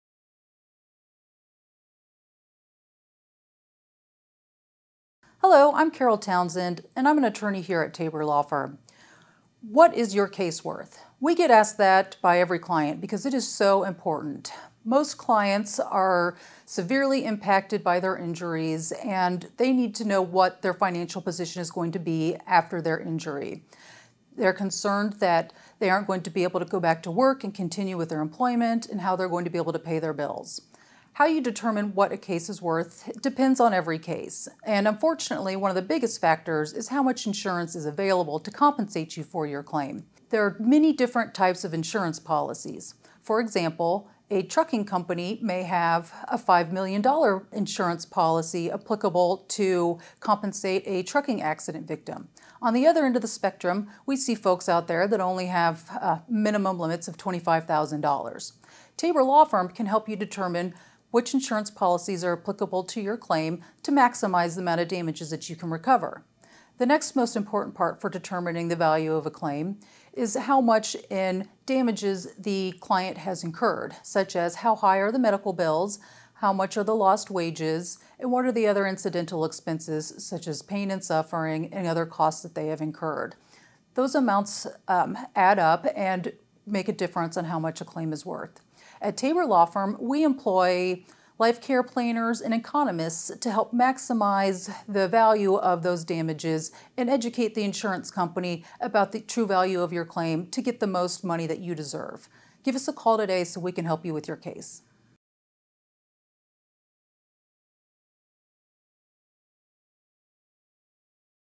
What’s My Personal Injury Case Worth?
Curious about the value of your personal injury case? In this video, Tabor Law Firm explains the factors that influence the worth of a personal injury claim, including medical bills, lost wages, and pain and suffering. Learn how to estimate your case’s potential value and what to expect in a settlement.